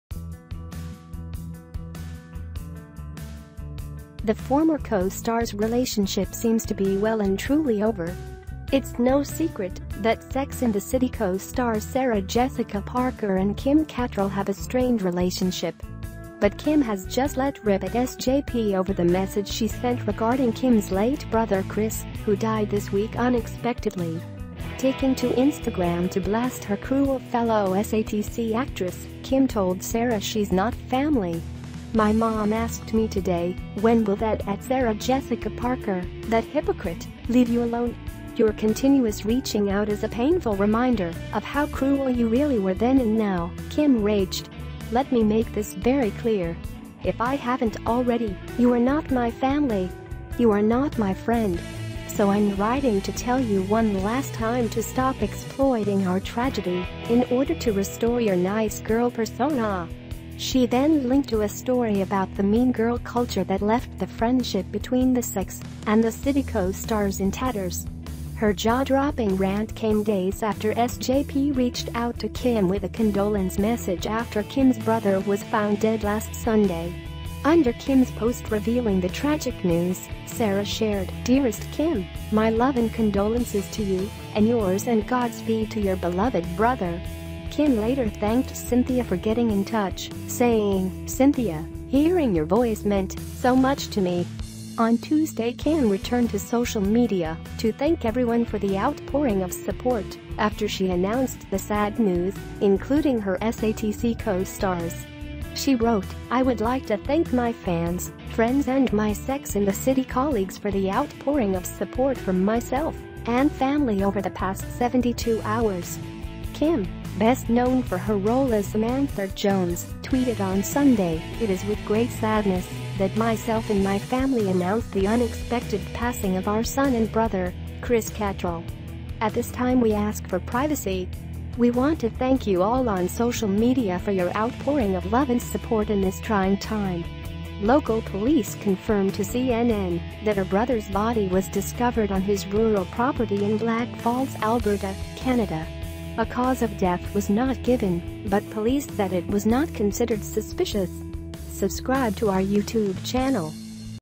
Kim Cattrall lashes out at 'cruel and hypocritical' Sarah Jessica Parker's reaction to her brother's
Kim Cattrall lashes out at 'cruel and hypocritical' Sarah Jessica Parker's reaction to her brother's death